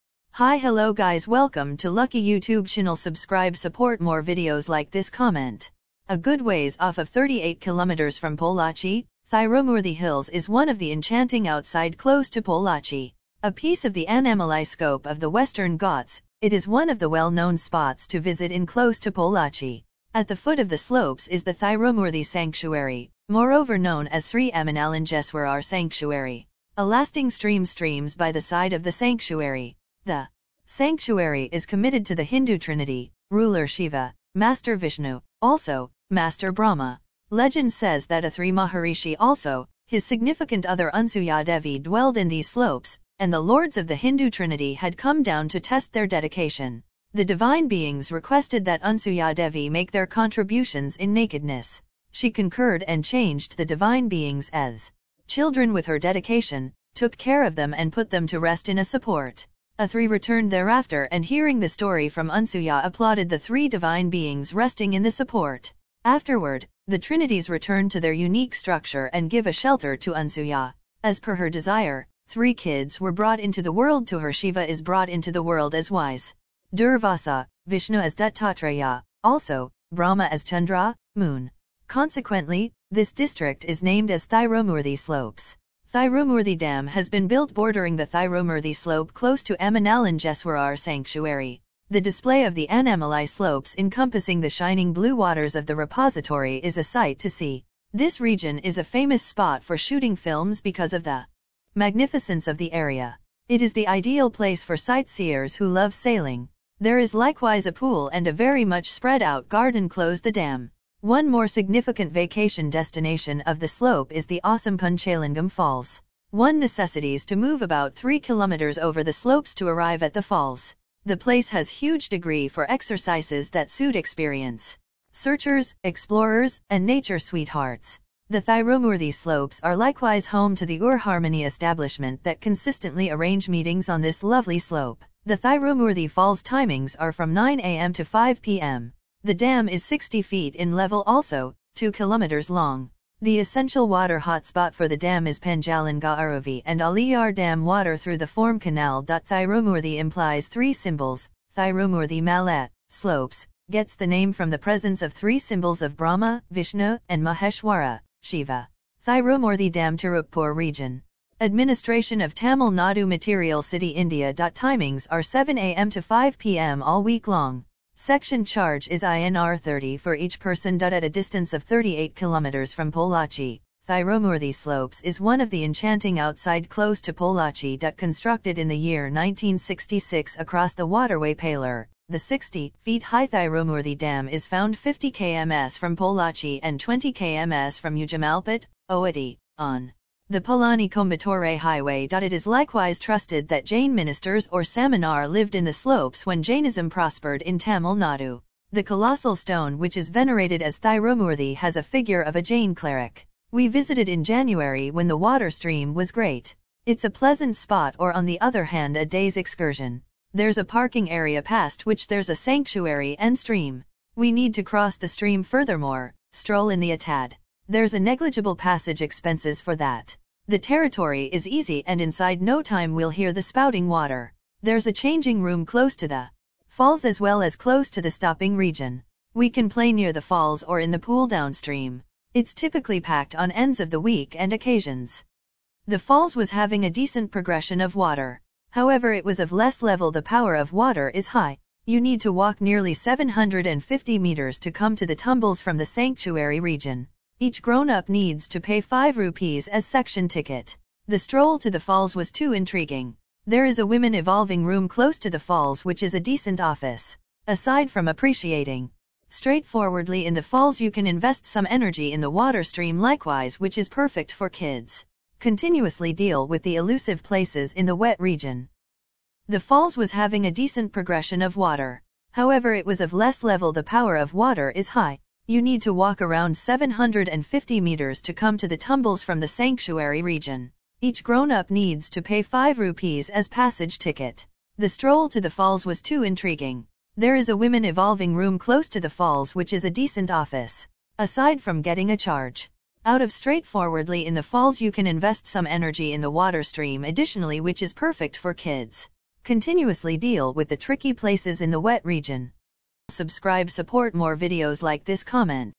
THIRUMOORTHY MALAI TEMPLE WATERFALLS VISIT REVIEW FEEDBACK ENGLISH | PART 1
The falls was having a decent progression of
water. However it was of less level
the power of water is high. You need to
walk around 750 meters to come to the
tumbles from the sanctuary region. Each grown-up
needs to pay five rupees as passage ticket.
The stroll to the falls was too
intriguing. There is a women evolving
room close to the falls which is a decent
office. Aside from getting a charge out of straightforwardly in
the falls you can invest some energy in
the water stream additionally which is perfect for
kids. Continuously deal with the tricky
places in the wet region